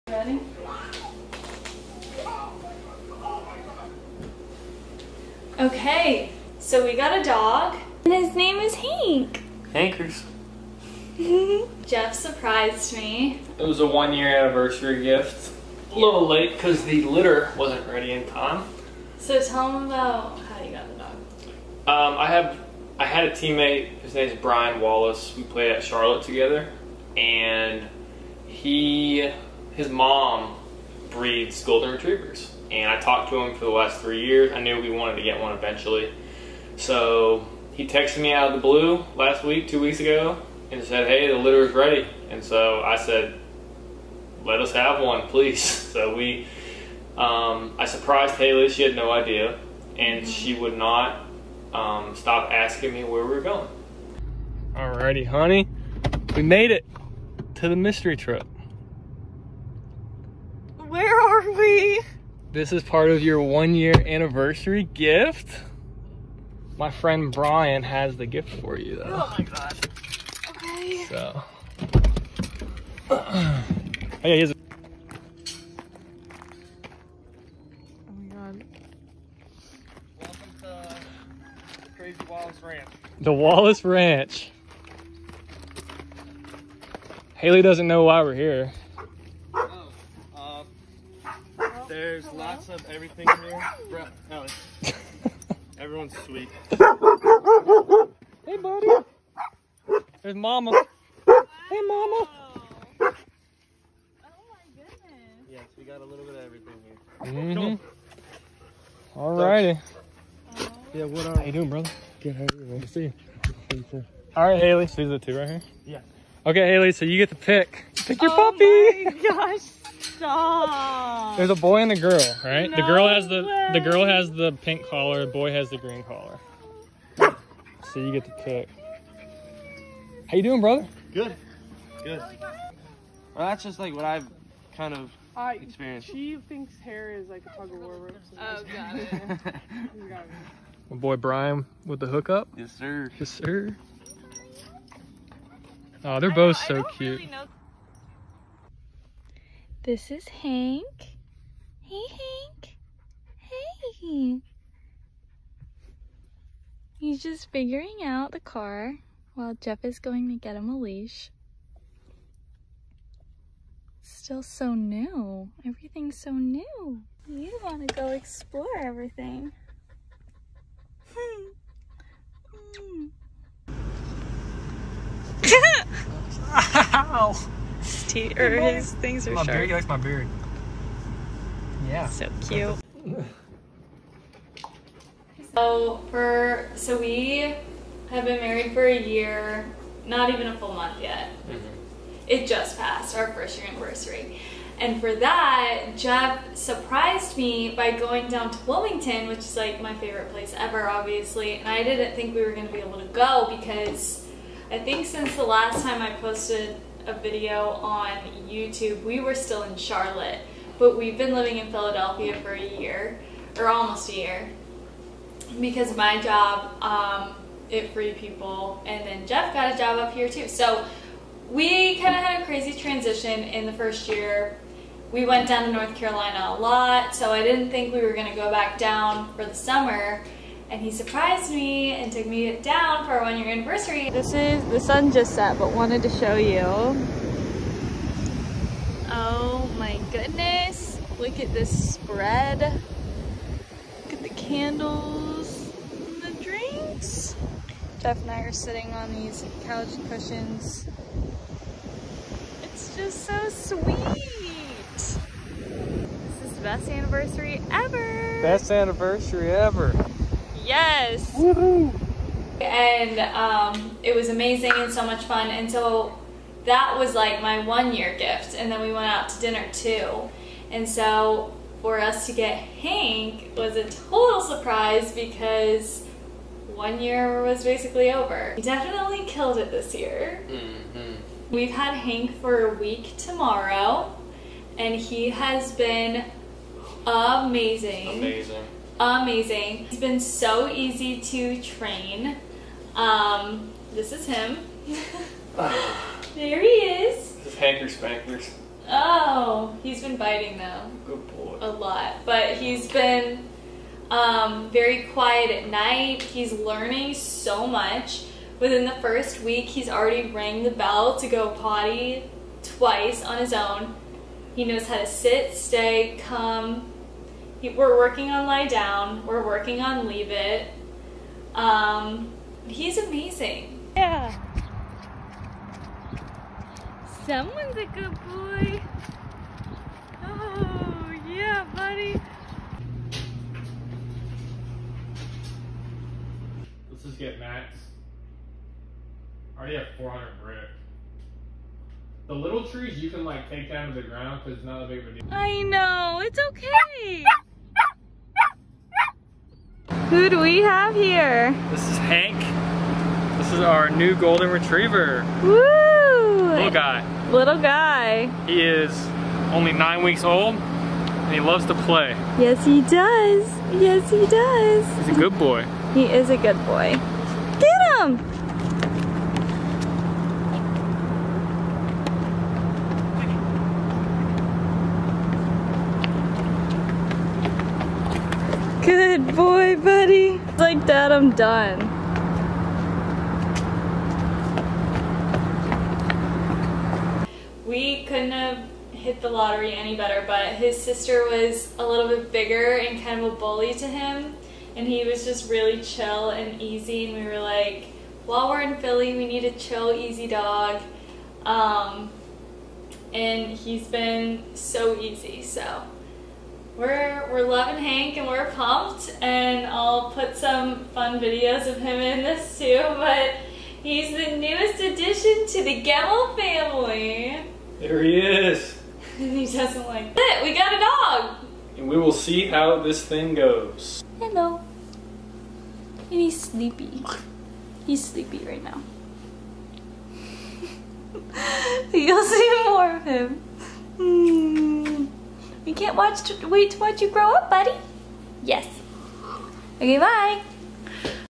MEET OUR GOLDEN RETRIEVER - HANK
Just got our first dog, hank! We love him so much already and cannot wait to watch him grow!